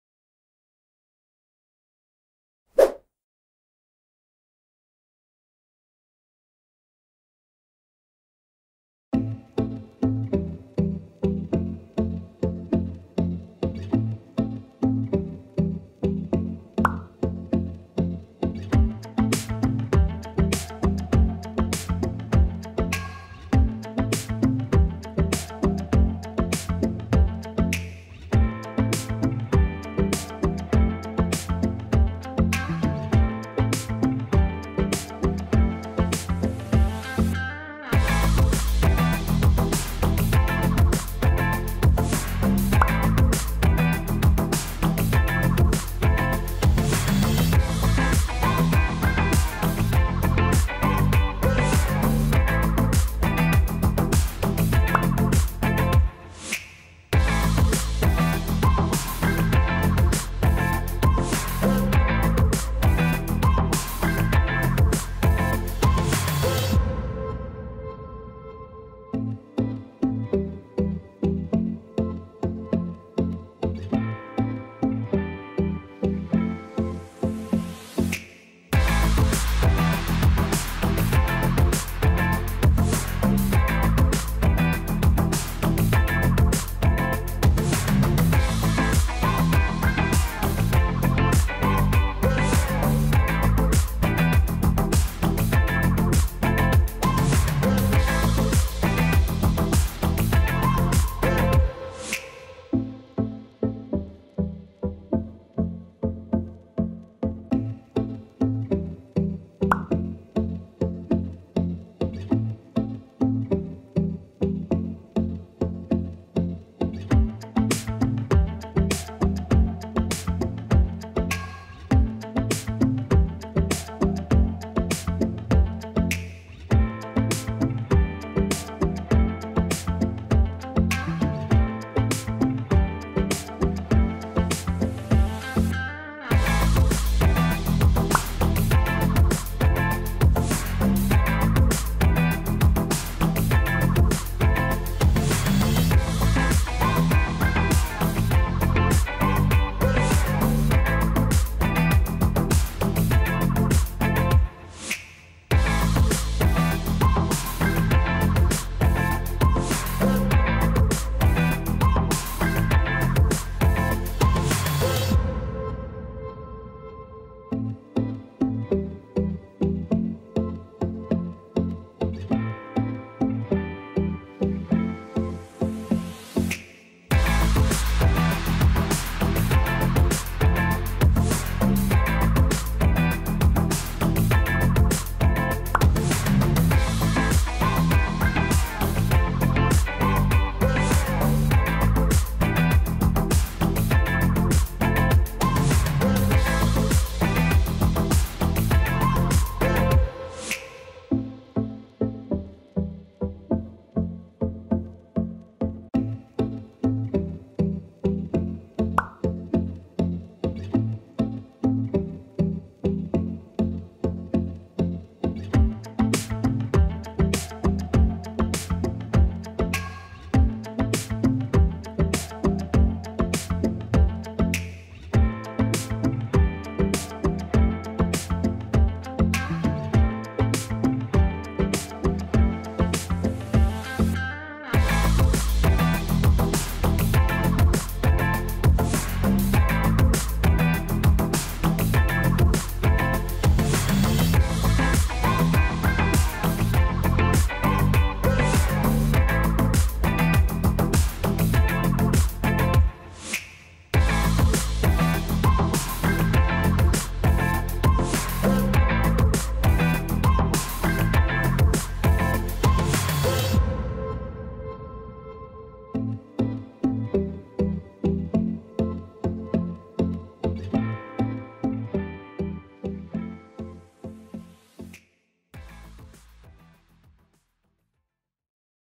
Cómo DAR CONTRASTE a una FOTOGRAFIA con SNAPSEED | Android - iPhone
Cómo dar contraste a una foto con SNAPSEED para verla mejor.